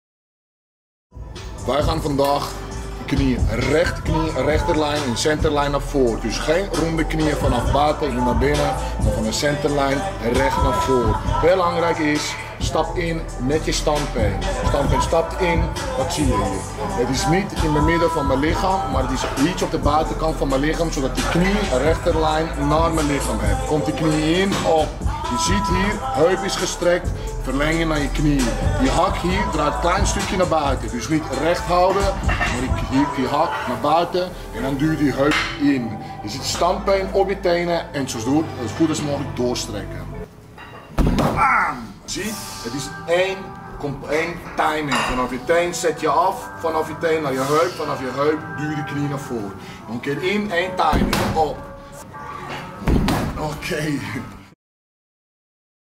Kickboks - directe knie
Een directe rechte knie! Bjorn Bregy en Robert de Leth doen het voor. Kijk je mee? 🙏🏻🔈